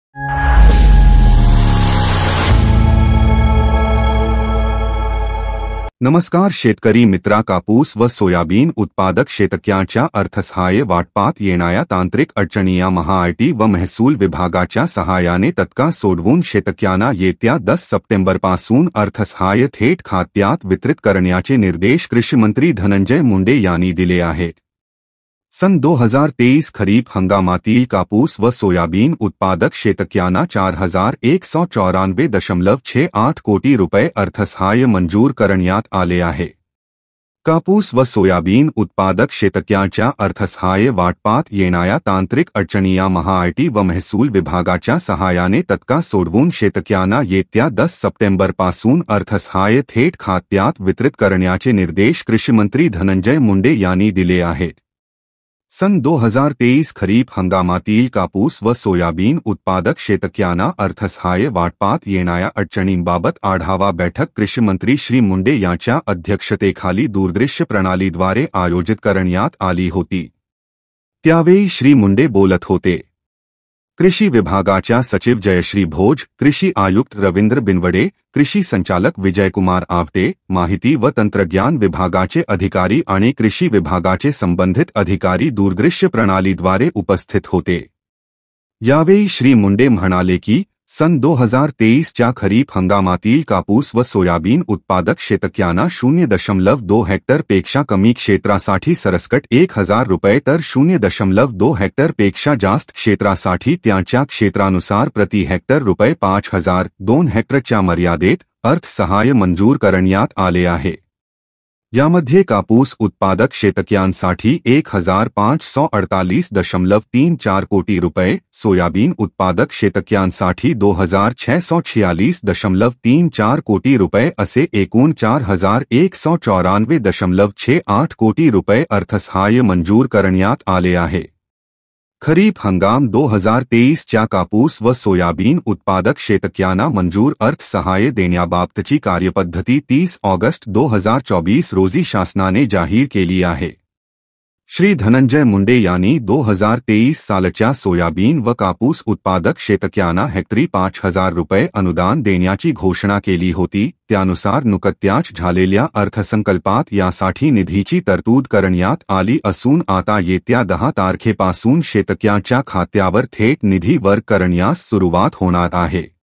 कापूस व सोयाबीन मदत थेट शेतकऱ्यांच्या खात्यात जमा करण्याची तारीख ठरलीkapusbhav
मानवत कापूस भाव आजचे | आजचे कापूस बाजार भाव |kapus bajar bhav today